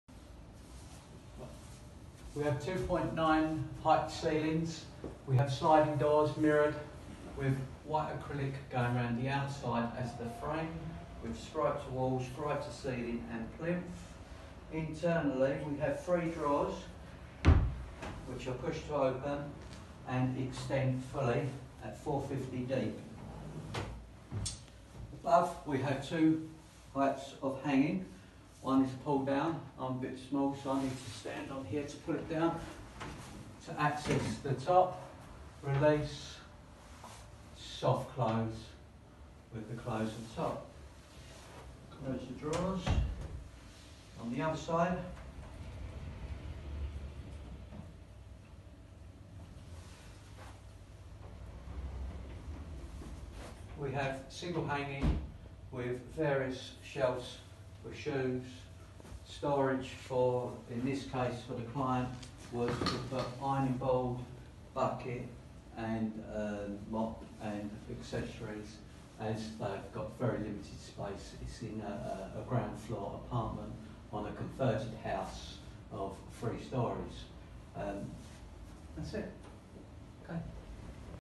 Tall sliding mirrored door wardrobe @ 3m height
3m tall sliding mirrored door wardrobe with profile handle and acrylic white surround, internal push to open drawers finished in Egger board Oak. to book a FREE design visit.